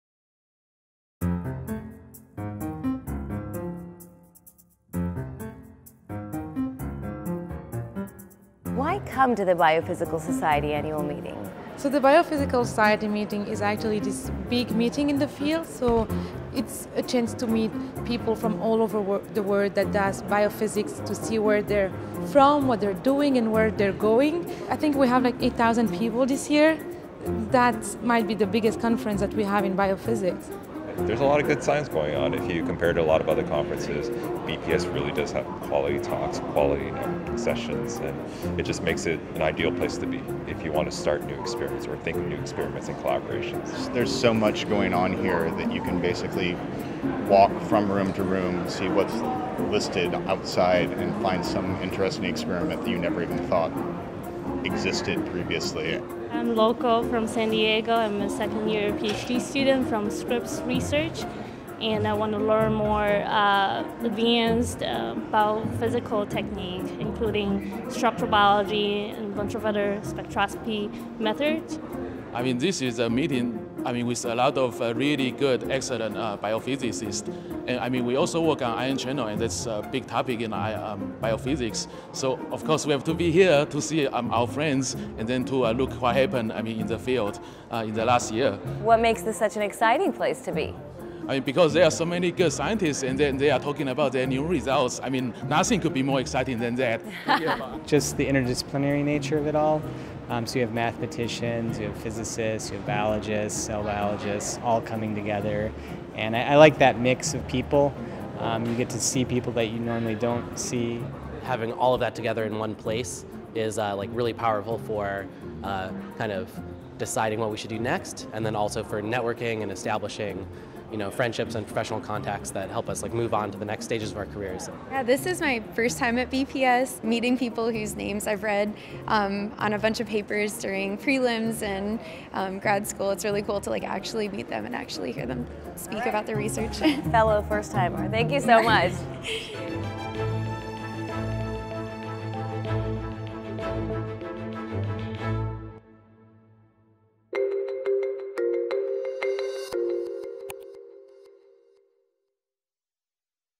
BPS20 and You - Why do you come to the Biophysical Society Annual Meeting?
We caught up with 2020 Biophysical Society Annual Meeting Attendees to hear what brings them to the meeting, and why it’s such an exciting place to be.